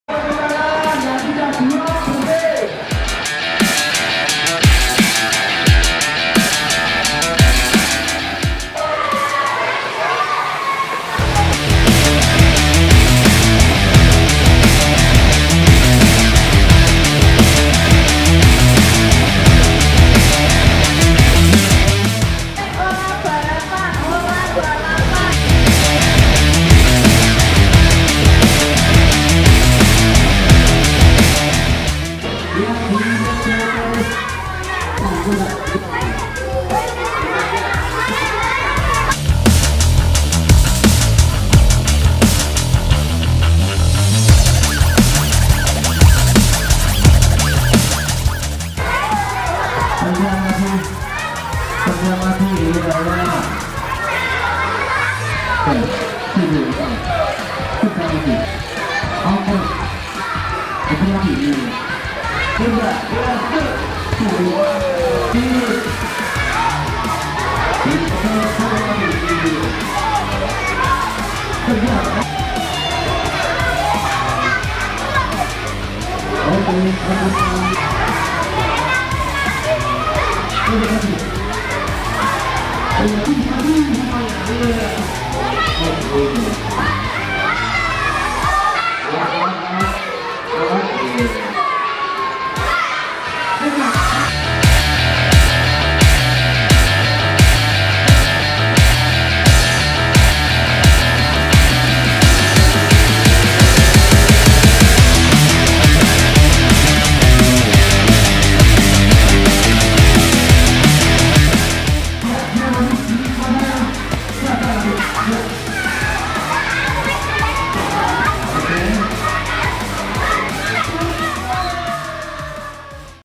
Permainan Bola Besar PJOK SD Kelas 2
PJOK Kelas 2 SD Ta'mirul Islam Surakarta : permainan bola besar.